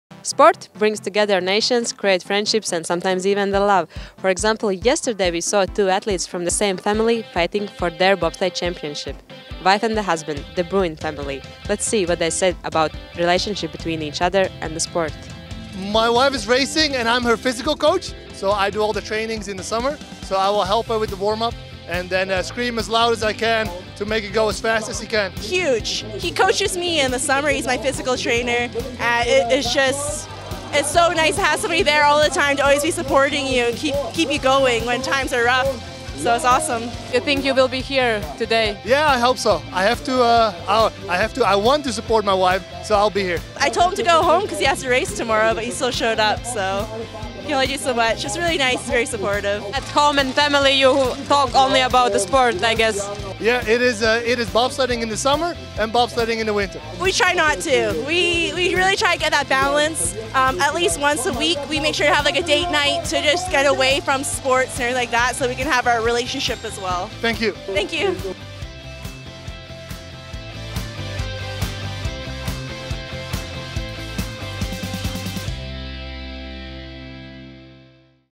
Family affair | BMW IBSF World Championships 2017
Love for sport, love for competition, love for medals. With Liva we discovered also the love between athletes. The De Bruin's family.